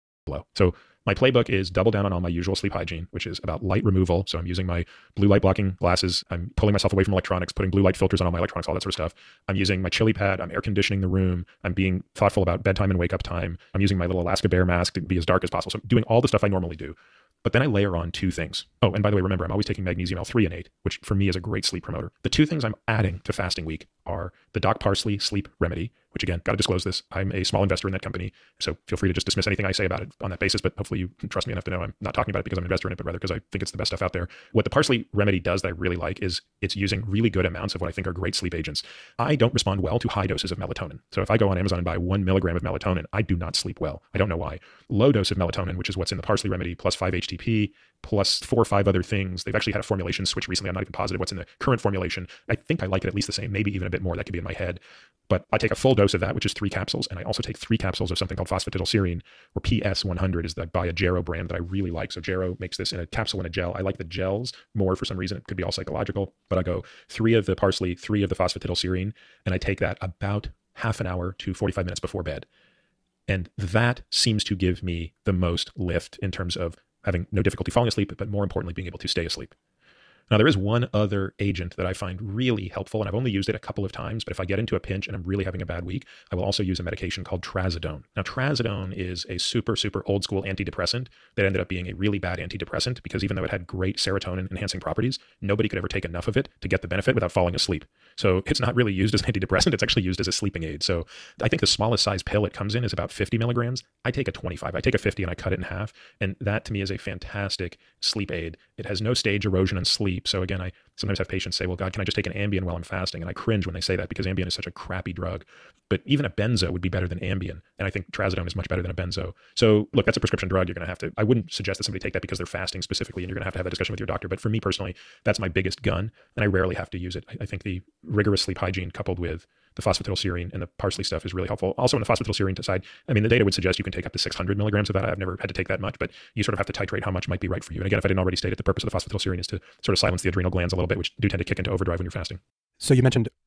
Peter Attia sleep playbook
[[Peter Attia]], a medical doctor with an obsession for self-experimentation and quantification, uses the following tools for better sleep: light removal, blue-light blockers, pulling away from electronics, filtering blue light from electronics, the Chilipad (and now Ooler), airconditioning the room, being thoughtful of sleep time and wakeup time, using the Alaska Bear mask, and taking Magnesium L-Threonate.

While fasting, [[Peter Attia]] adds the following to his sleep routine. He takes 3 capsules of Doc Parsley sleep remedy, which contains a low dose of [[Melatonin]] plus 5HTP and 4 or 5 other ingredients. He also takes 3 capsules of PS100 (Phosphatidylserine). This silences the adrenal glands, which tend to be more active when fasting. He does these 30-45 minutes before bed. On rare occasions, he brings out his biggest gun: 25mg of Trazaodone, which is a prescription drug. Unlike other pharmacologic sleep aids, Peter Attia says that this drug does not erode his sleep stages, as measured by his sleep tracker, the Oura ring.